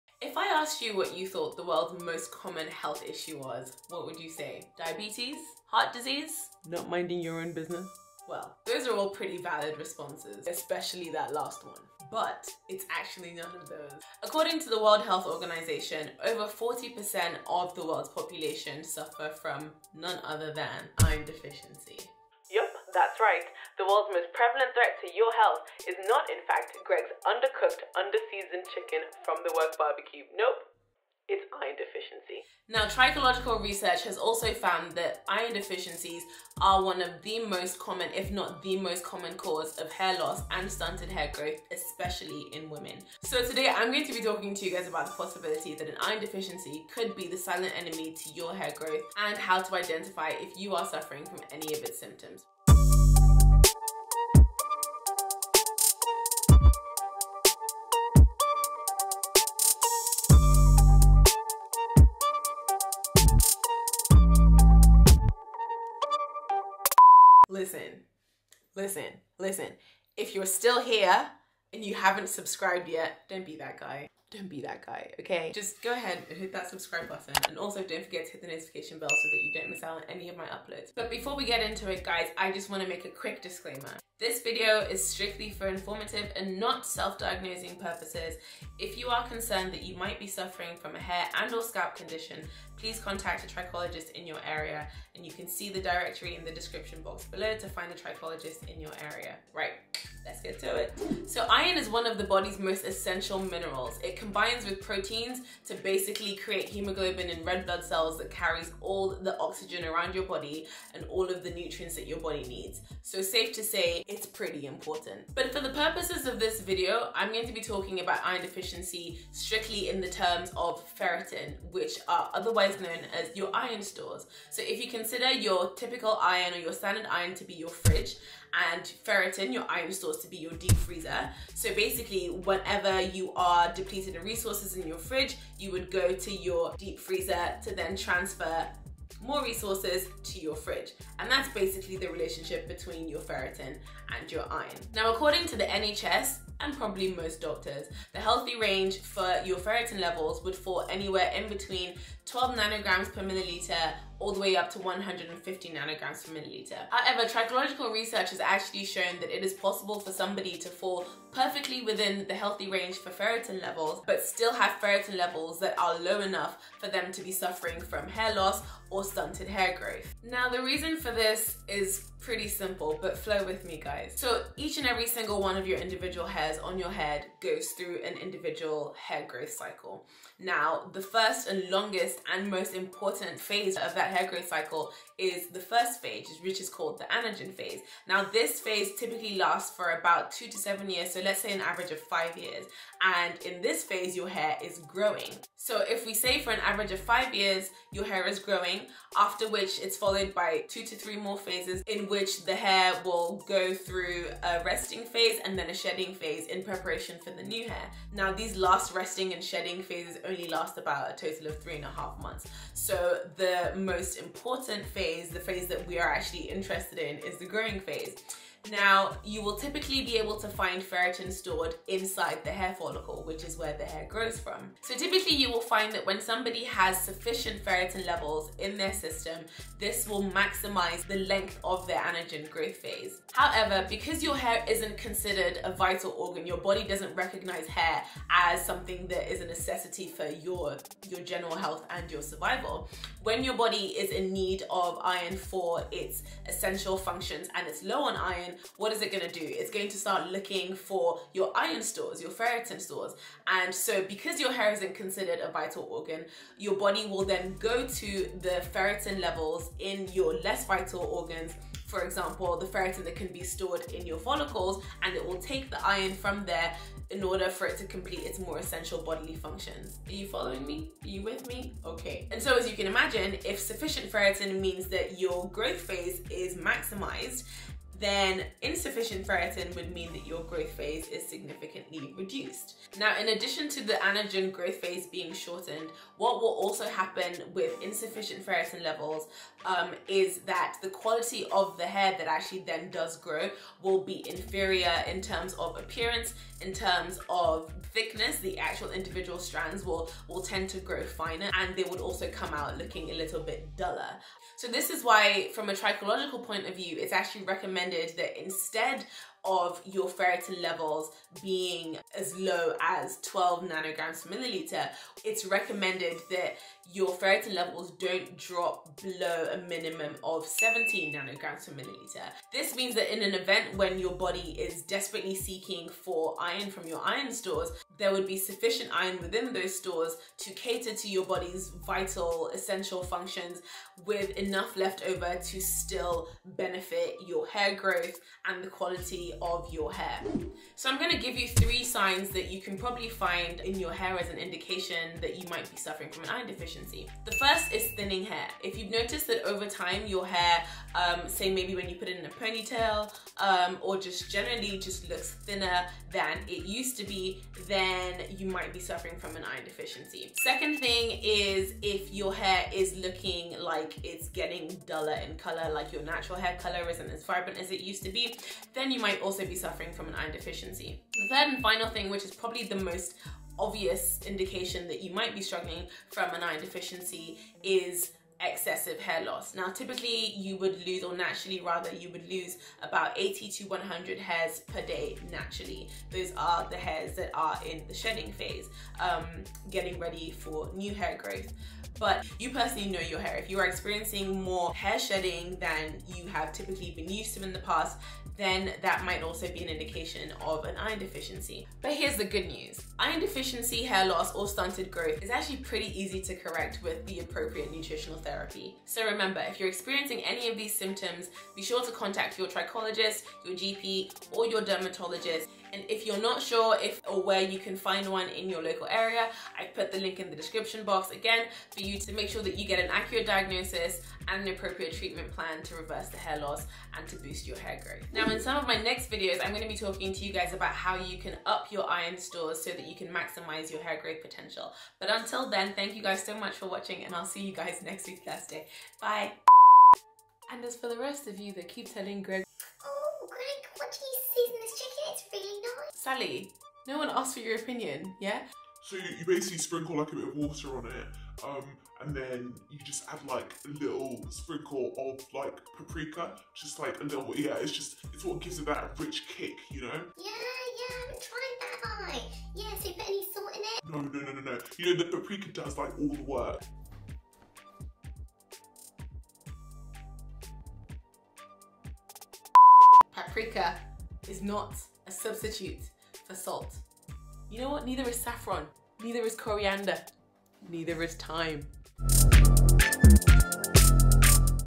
The REAL Reason Your Hair Isn’t Growing! - Iron Deficiency
Yup. That’s right

...An iron deficiency is the most common health condition in the world, and could very well be the underlying reason your hair isn’t “growing” or why it's shedding excessively.

If you identify with any of the symptoms I talk about in this video then do me a favour…Don’t self-diagnose.